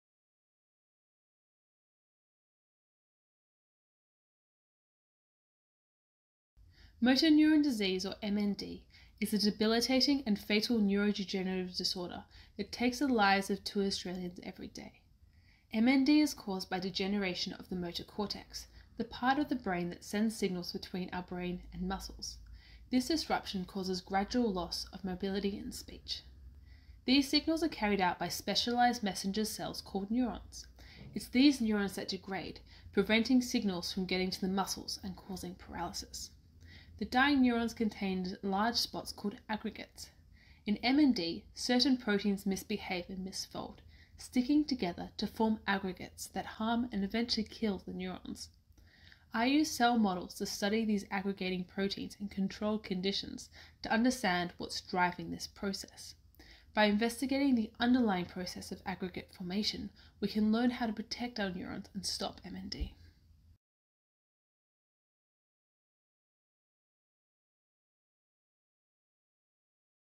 Visualise Your Thesis 2023 - Elise Kellett - Misbehaving problems in MND
Entry in the 2023 Visualise Your Thesis competition at The University of Queensland – “Misbehaving problems in MND” by Elise Kellett – ORCID Attribute this work under an Attribution 4.0 International Licence (CC BY 4.0)